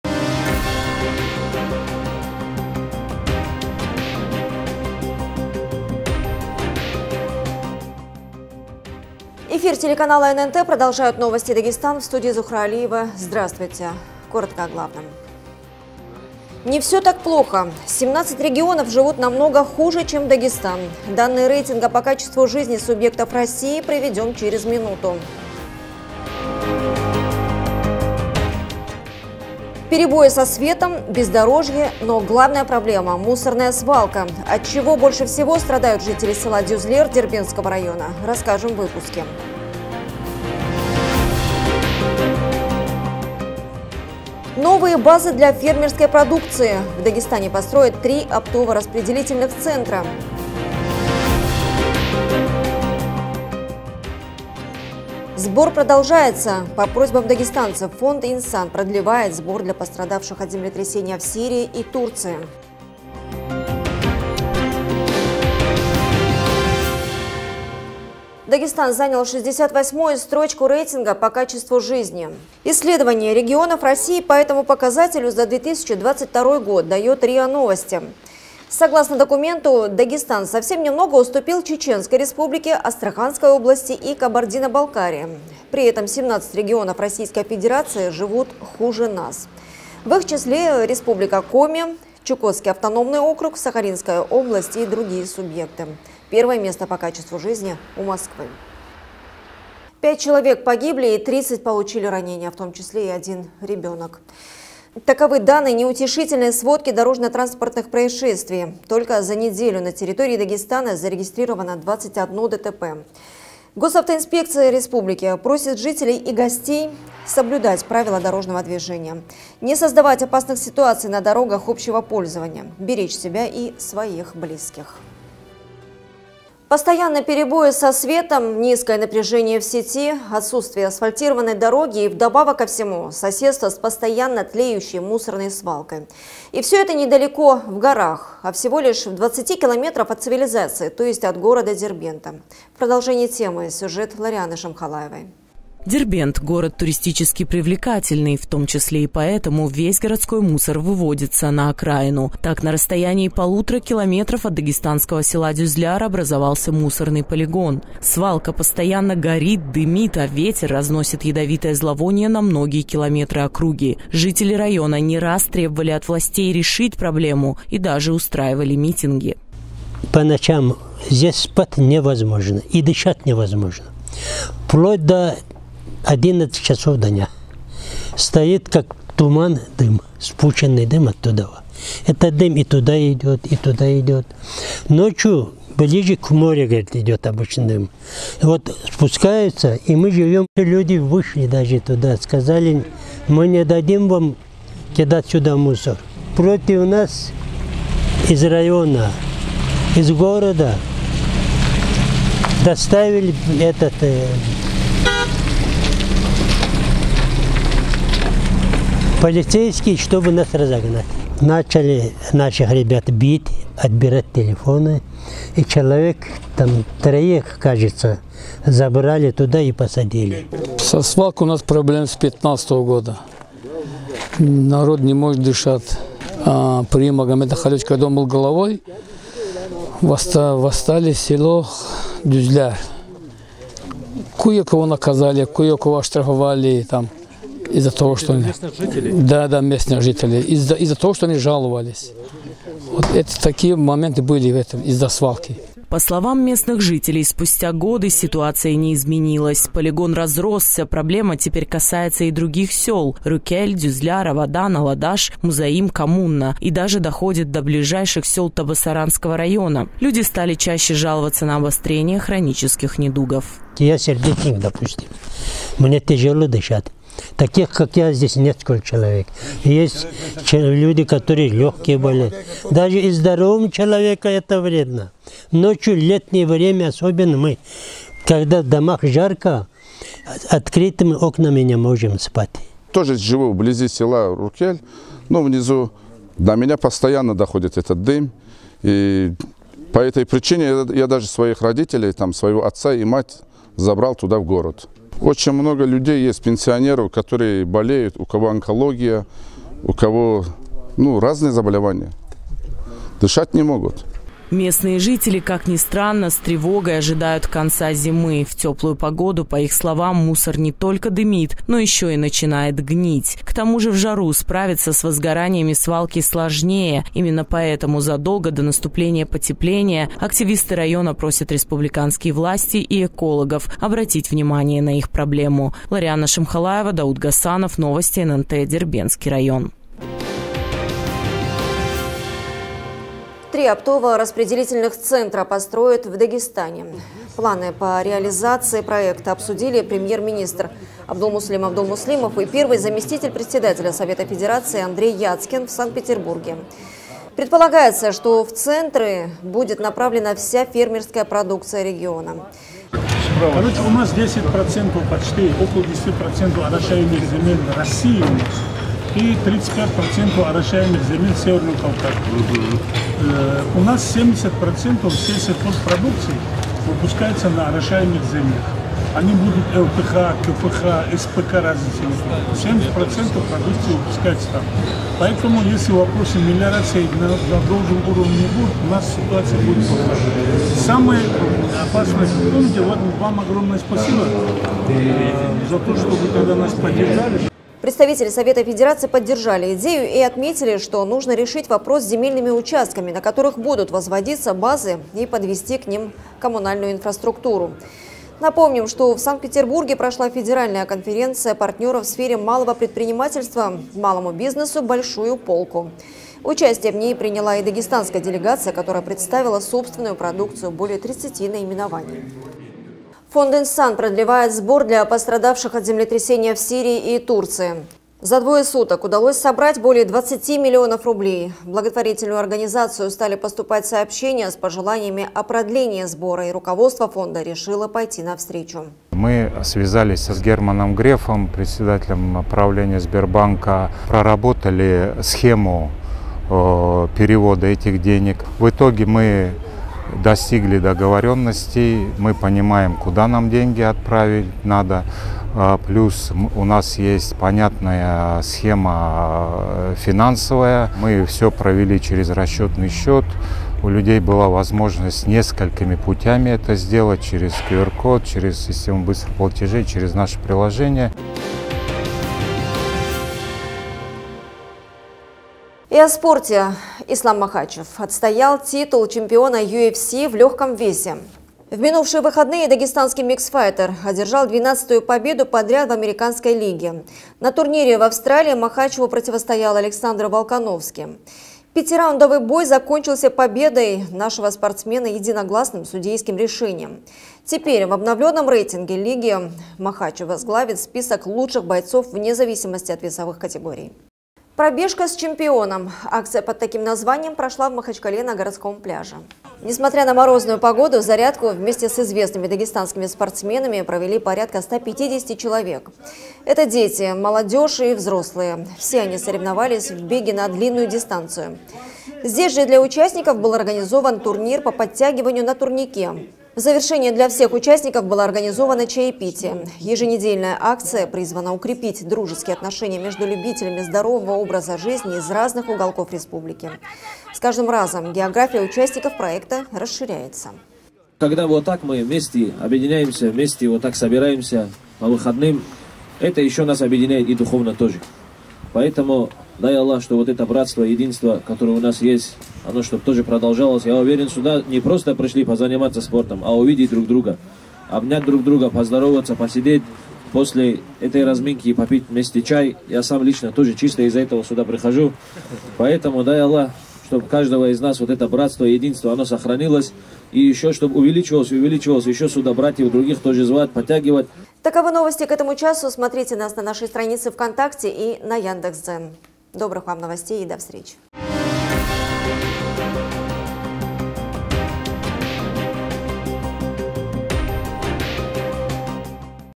Новости Дагестана за 13.02.2023 год
Не все так плохо. 17 регионов живут намного хуже, чем Дагестан. Данные рейтинга по качеству жизни субъектов России приведем через минуту.

Перебои со светом, бездорожье, но главная проблема мусорная свалка. От чего больше всего страдают жители села Дюзлер дербентского района, расскажем в выпуске.

Новые базы для фермерской продукции. В Дагестане построят три Оптово-распределительных центра.

Сбор продолжается. По просьбам дагестанцев Фонд «Инсан» продлевает сбор для пострадавших от землетрясения в Сирии и Турции.